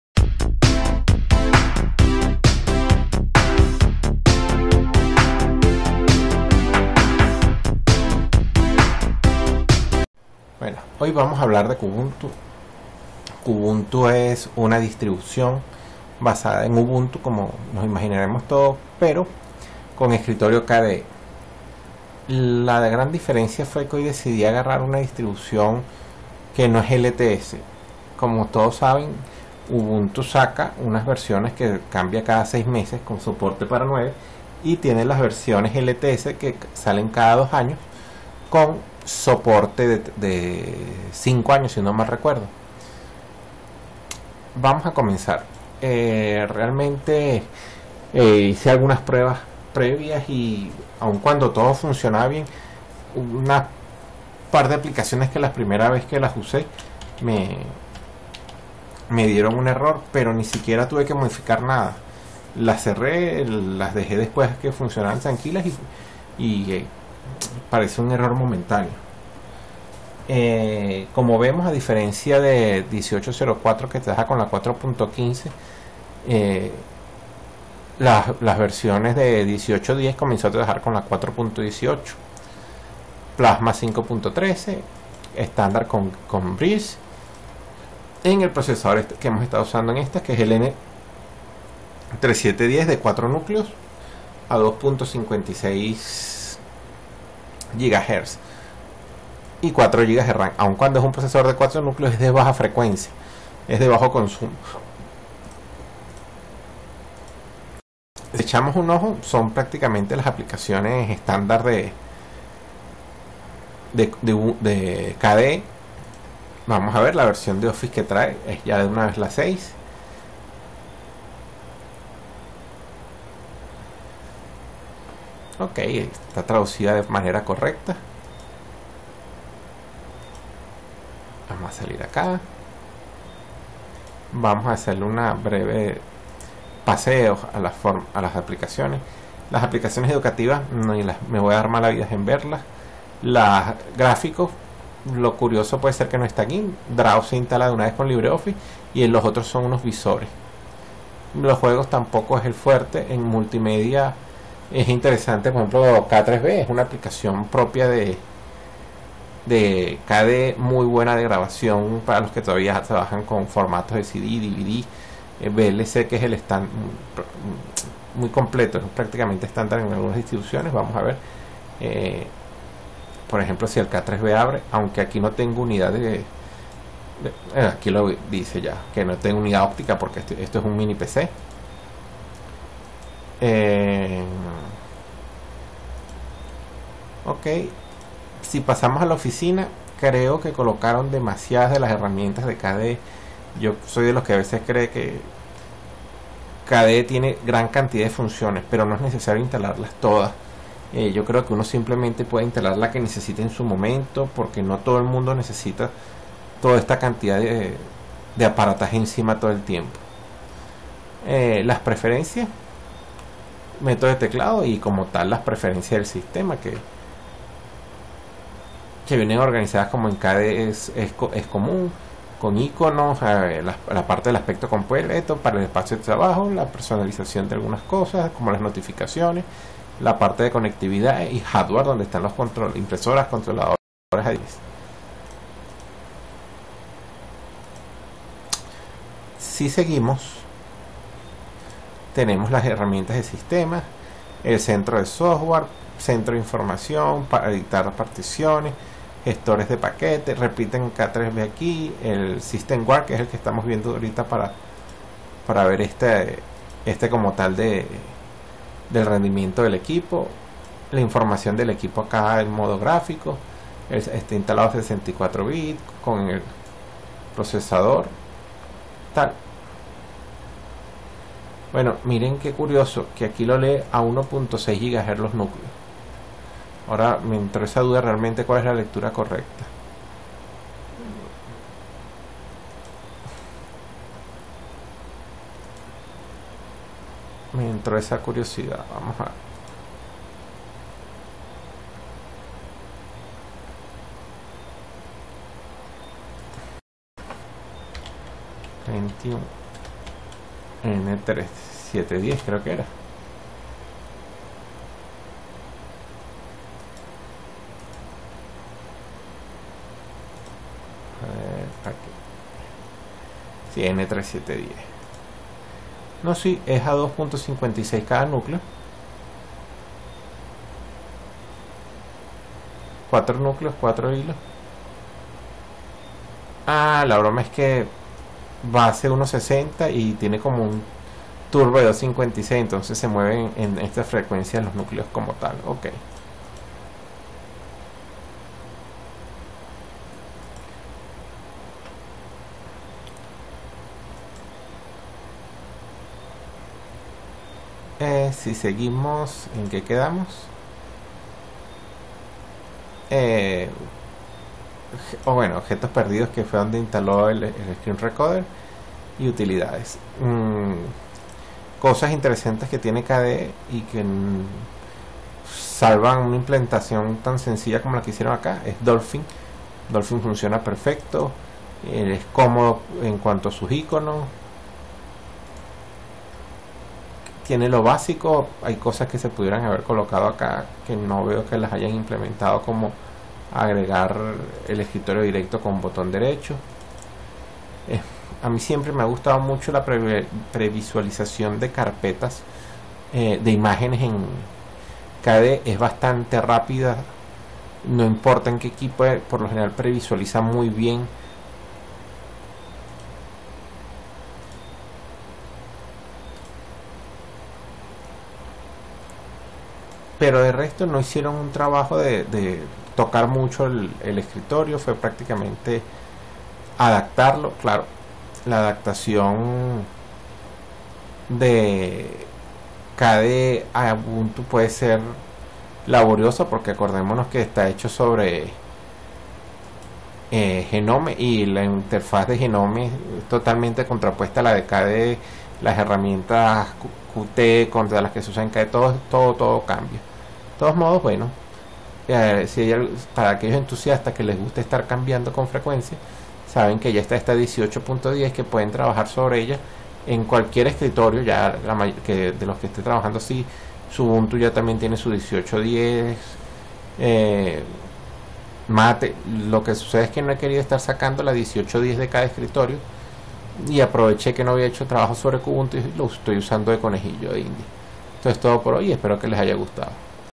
Revisión Kubuntu 18.10 Review Español
Kubuntu es el sabor de Ubuntu donde se utiliza a KDE como escritorio, aquí con su versión no LTS con un ciclo de 6 meses, que sirve de banco de prueba para los entusiastas que desean ir viendo el progreso de la distribución en los períodos intermedios de las LTS.

Un trabajo fuerte se debe realizar para adaptar KDE a Ubuntu distribución con librerías diseñadas para Gnome en su origen y que deben ser adaptadas para dicho escritorio.